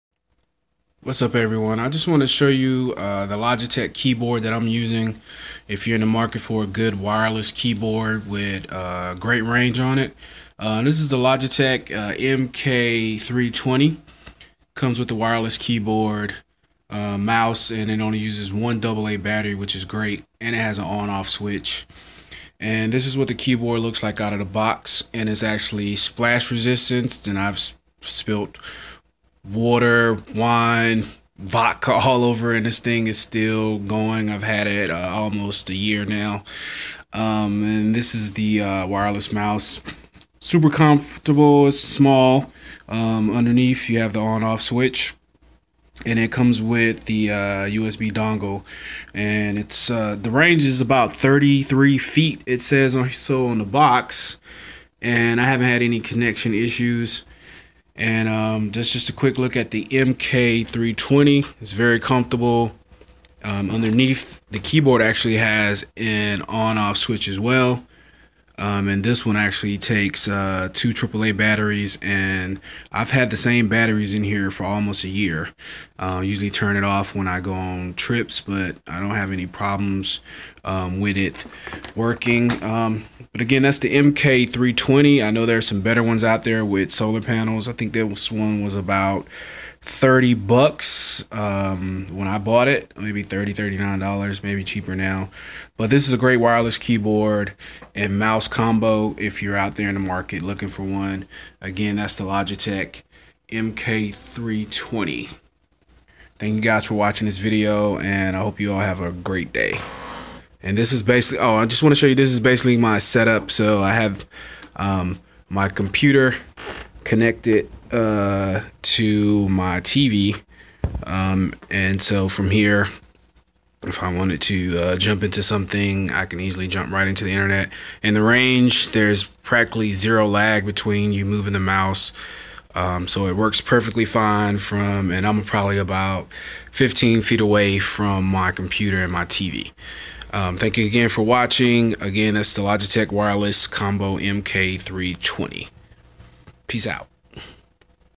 Wireless Keyboard & Mouse
Logitech MK320 water resistant keyboard & mouse. It's $24.99 on Amazon Right Now